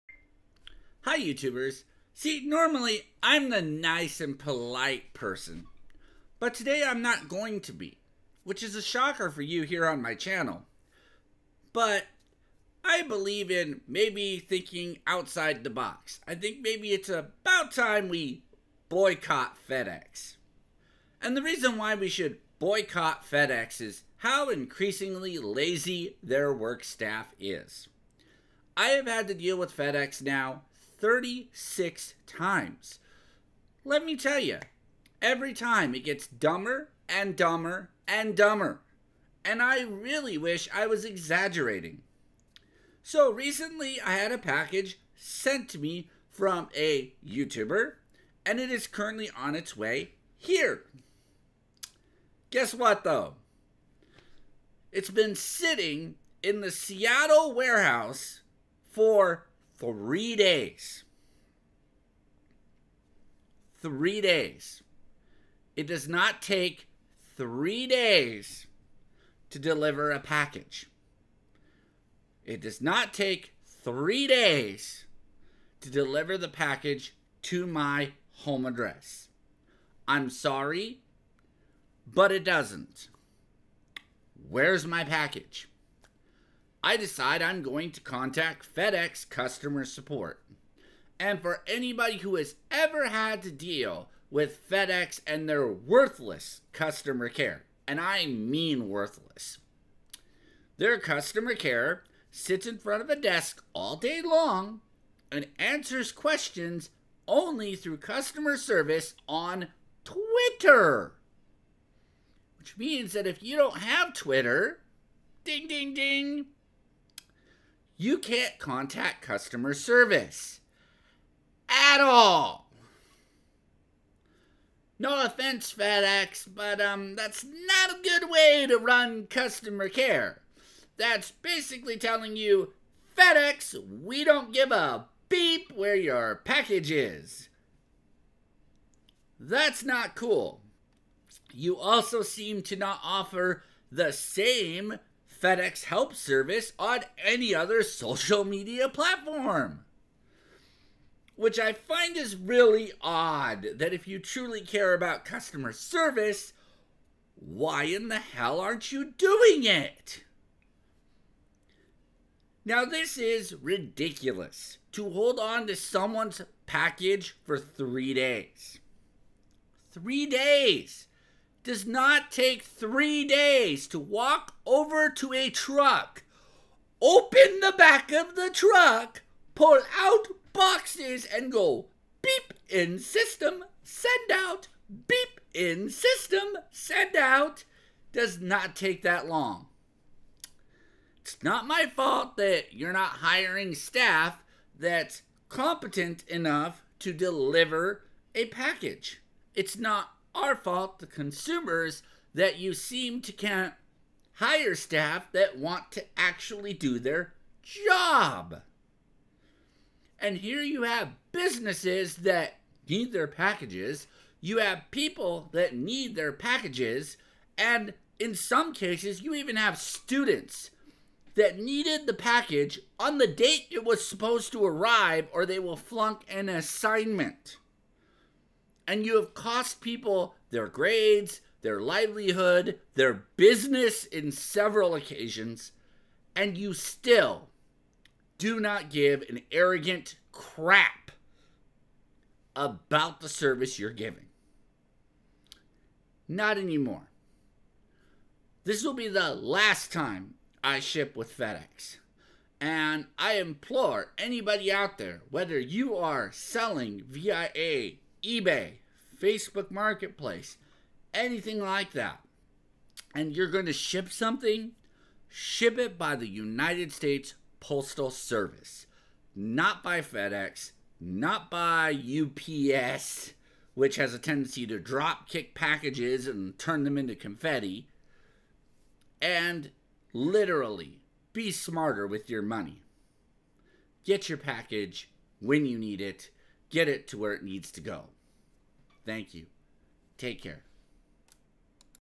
Time To Boycott FEDEX How much lazy is to much
So this is the 36th time FedEx has failed to deliver a package on time im for fully boycotting Fed Ex i think there services suck there staff cant properly process a package they lie and say weather is a issue when its sunny out and blue skys there customer service seems to only work through twitter a lot of bad decisions that dont help customers please comment below share with people i want to know have you had to deal with fed ex lieing to your business or students or other cases post comments down below